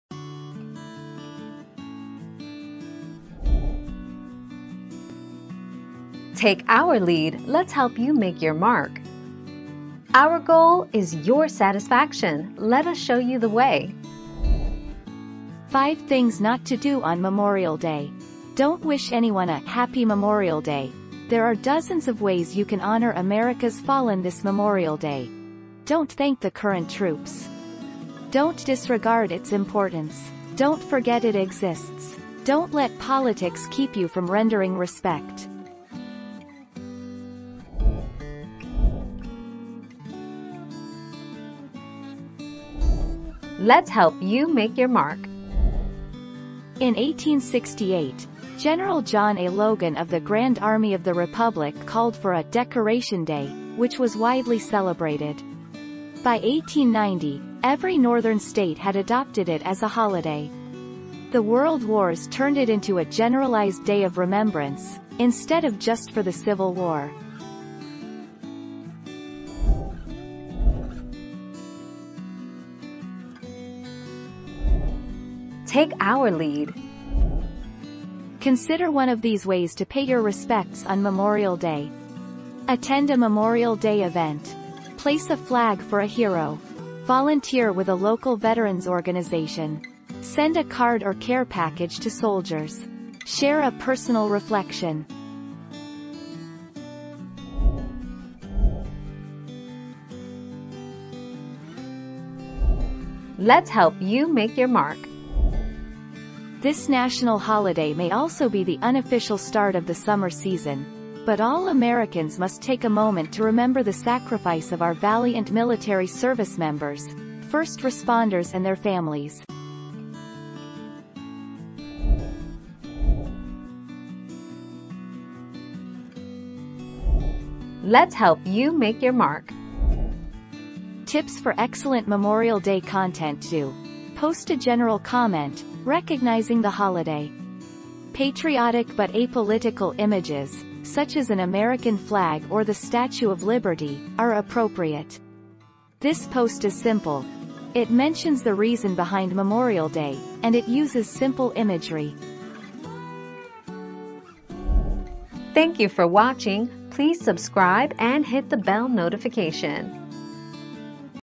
What should you not do on Memorial Day?
00:00 - What should you not do on Memorial Day?
00:39 - How did Memorial Day really start?
01:11 - How do you pay respect on Memorial Day?
01:39 - Is Memorial Day for first responders?
02:03 - What is a good Memorial Day post?

Laura S. Harris (2021, May 27.) What should you not do on Memorial Day?
    AskAbout.video/articles/What-should-you-not-do-on-Memorial-Day-248865